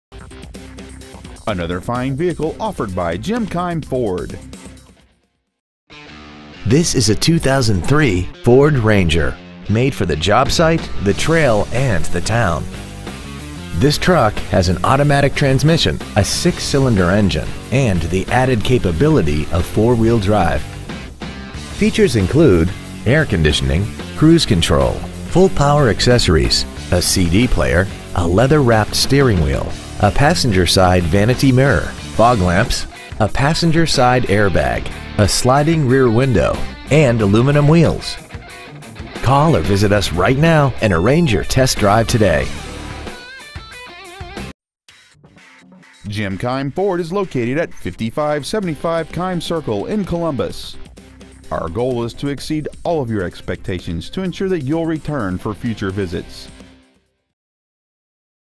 2003 Ford Ranger Columbus OH
Year : 2003
 Make : Ford
 Model : Ranger
 Engine : 4L 6-Cylinder
 Trans . : automatic
 Exterior : Oxford White
 Miles : 90,269
 Interior : Ebony

 Pre-Owned 2003 Ford Ranger Columbus OH